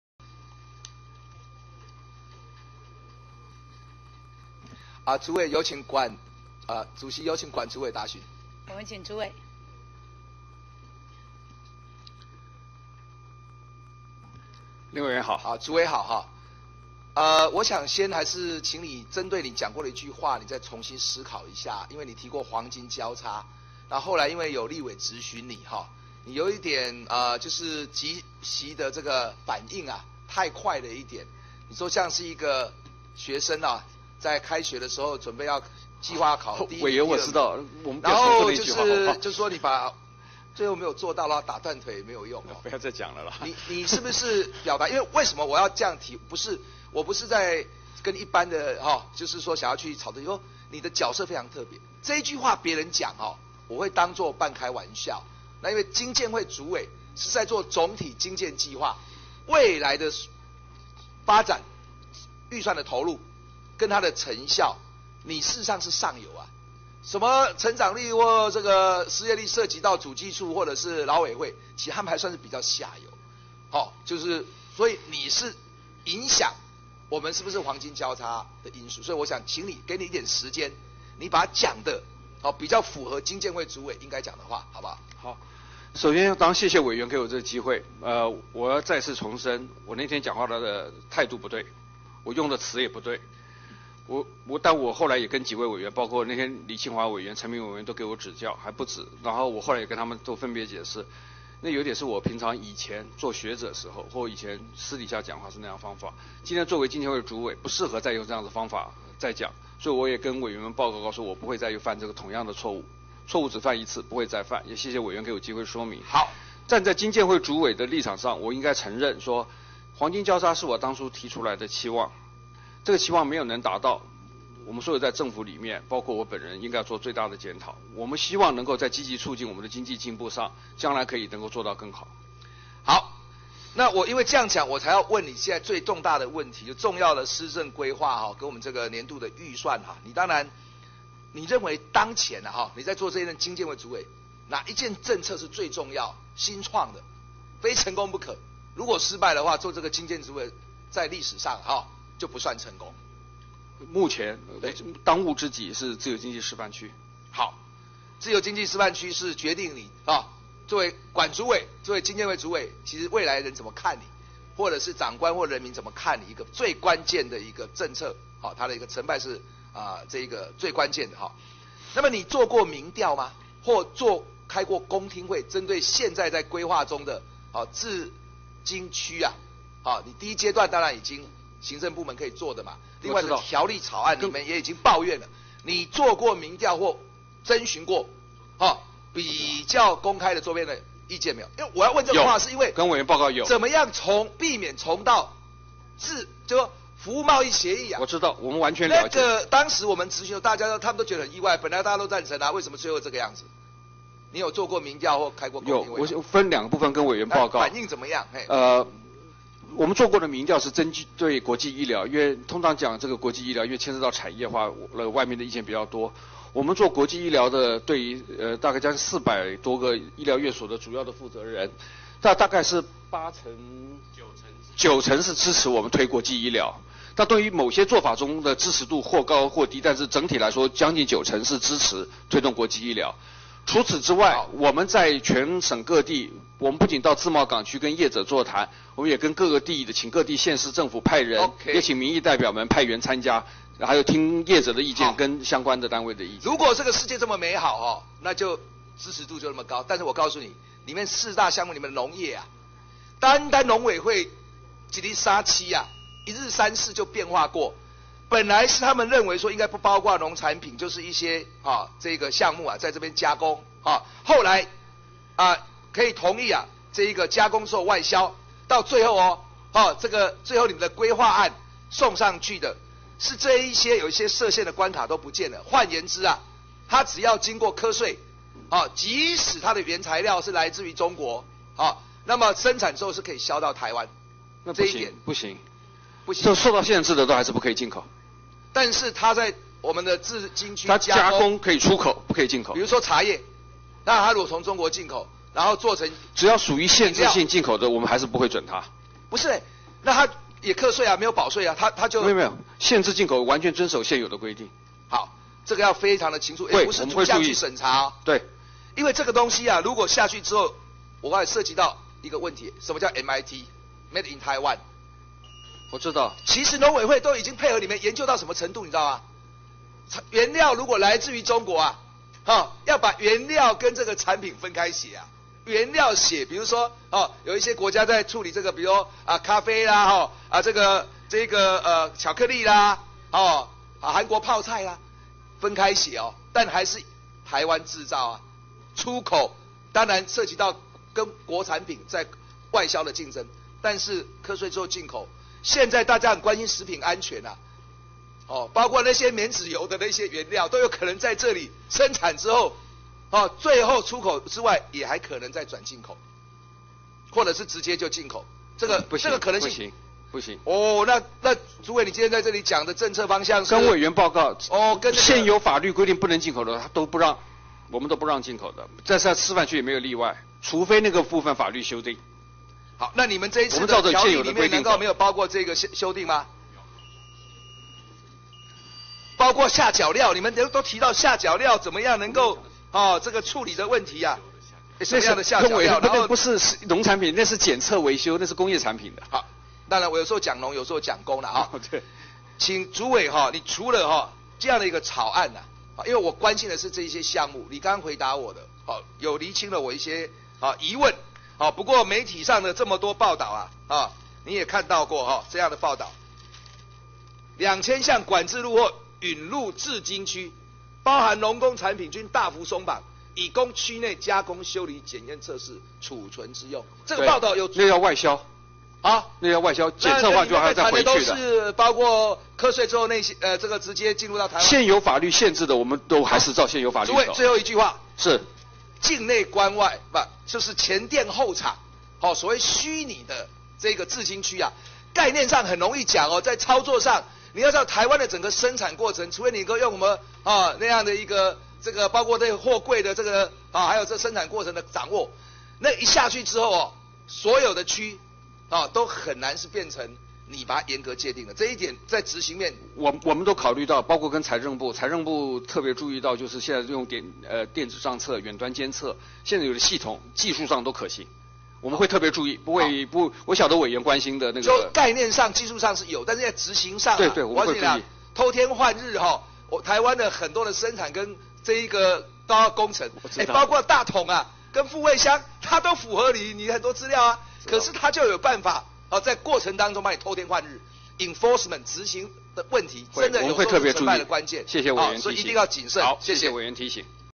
20131028 林佳龍：自經區到底能不能進口管制性中國貨品 政策必須說清楚
立法委員林佳龍今日在經濟委員會指出，根據媒體報導，針對兩千多項管制性中國農、工產品，行政院打算透過自由經濟示範區特別條例進行鬆綁，未來，這兩千多項管制性貨品將可輸入自經區，以供區內加工、修理、檢驗、測試、儲存之用。此外，特別在902項管制性農產品的部分，原先都說只能加工後全數外銷，但現在傳出農委會又可能以「逐一審查把關」的方式，讓加工後的成品在完稅後可以進入台灣的國內市場。林佳龍強調，這樣等同於全面開放，與自經區的原始規畫內容差距太大，這部分，經建會有義務向國人清楚交代。

對此，經建會主委管中閔表示，只要屬於管制性產品，一律不准輸入國內，只能在加工後全數外銷。管中閔強調，除非現行法令規定修改，否則即便有特別條例，自經區也不會有例外，就是完全不能進口。此外，管中閔也針對日前失言風波再次作出澄清，他說，先前會以「難道考不到第一名，就要打斷腿嗎」的說法回應「黃金交叉」跳票一事，不僅說法不對，態度也不對，因為沒有達到「黃金交叉」這個政策期待，政府內部包括他自己，都要做深刻的自我檢討，並且找出可以做得更好的方法。

其次，針對林佳龍詢問自經區政策制定過程中，是否有辦過公聽會或做過民調，以進行更廣泛的意見徵詢。管中閔則指出，曾就「國際醫療」這個部分與400多所醫療院所的負責人有過溝通與調查，其中有高達9成的反應都屬正面，但其他的部分，則尚未進行。